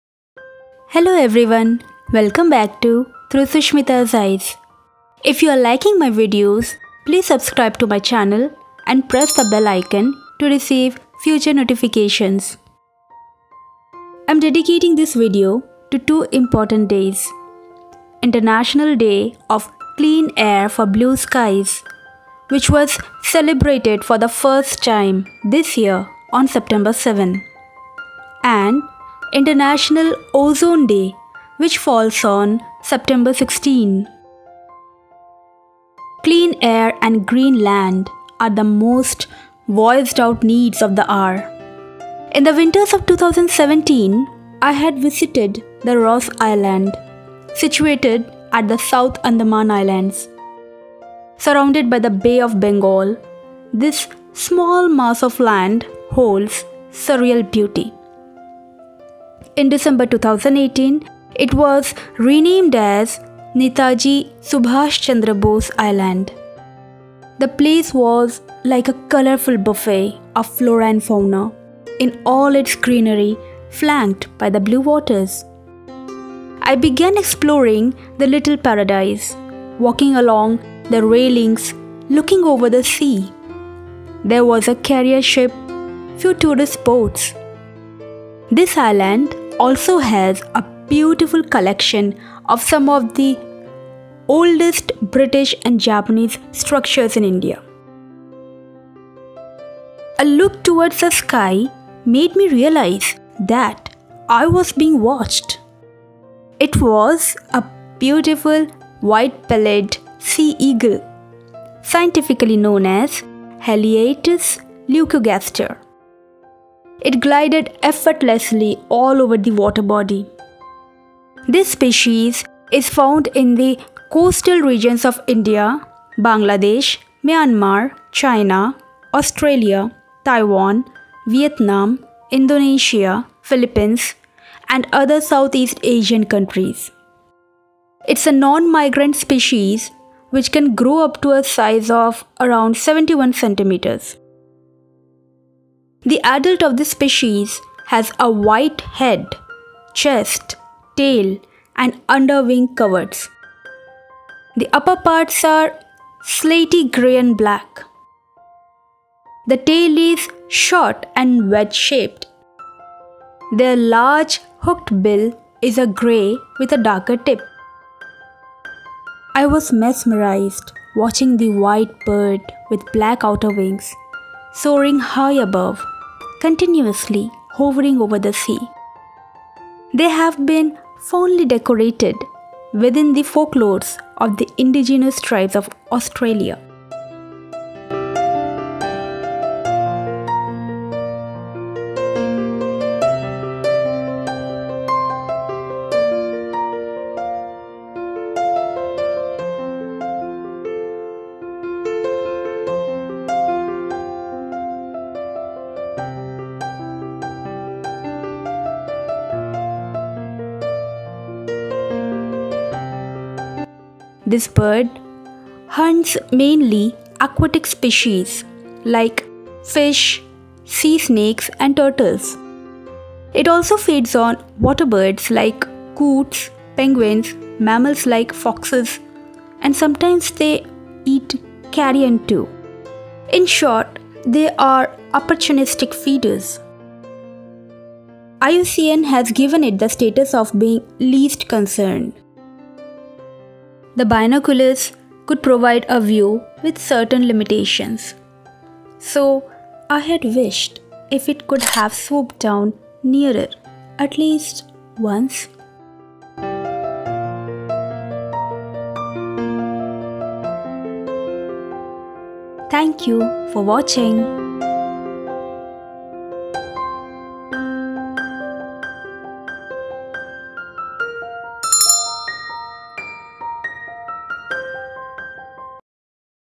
Watercolour painting of White-bellied Sea-eagle
Presenting a video of painting the White-bellied Sea-eagle. This majestic species of family Accipitridae, was real fun. I loved painting the shiny feathers, which were a mixed shade of brown, black and grey. The incident described, is the first time I had spotted an eagle.